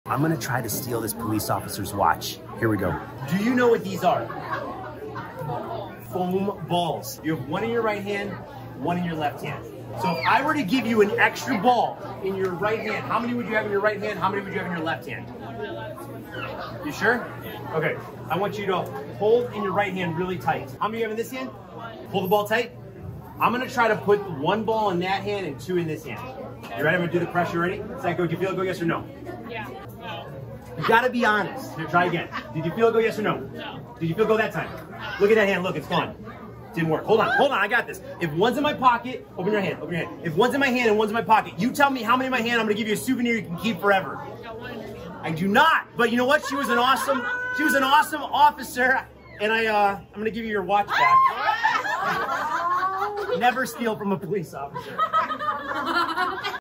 Stealing A Police Officers Watch 👮‍♀️
How did this police officer not notice I STOLE HER WATCH? ⌚️